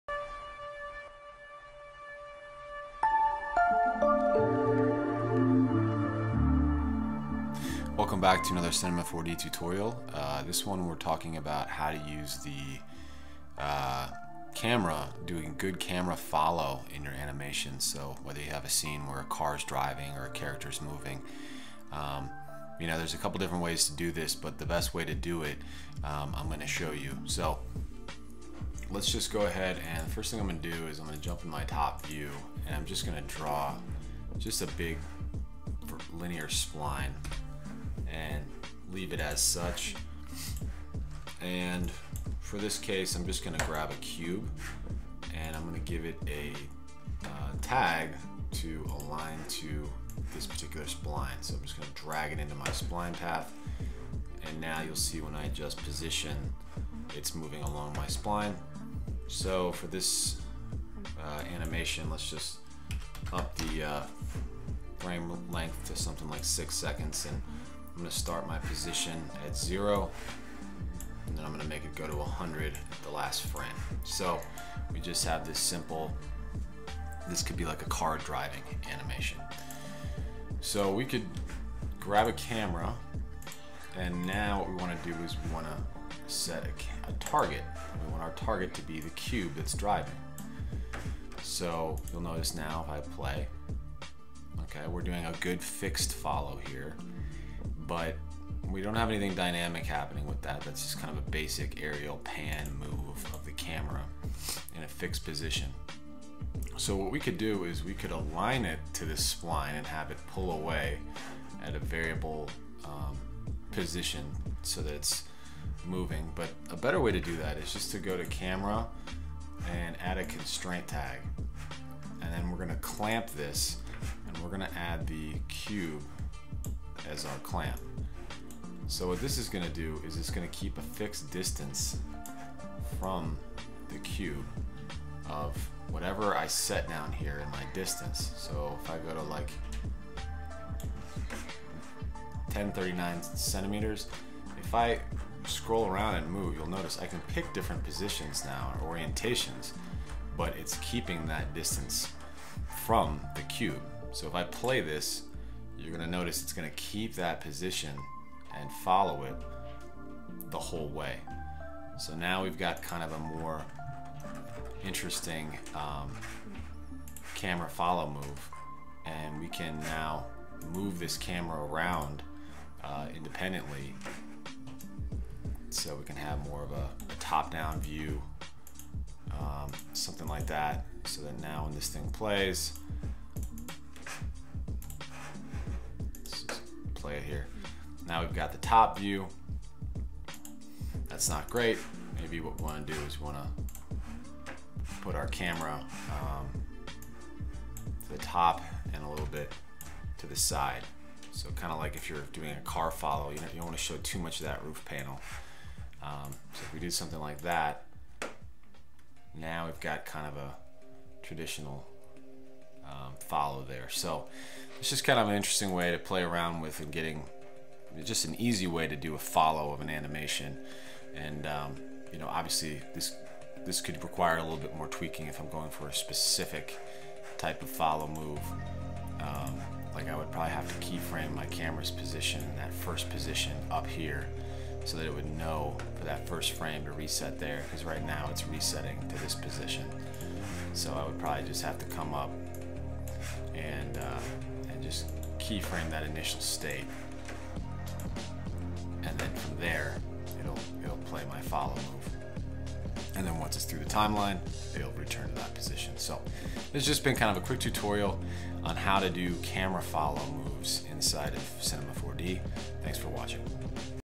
How to do camera follow in Cinema 4D
In this tutorial, we show you how to do a good camera follow move making it animated in Cinema 4D using the target and constraint tag. This is an introduction to setting up the camera with tags to interact with your objects for animation purposes.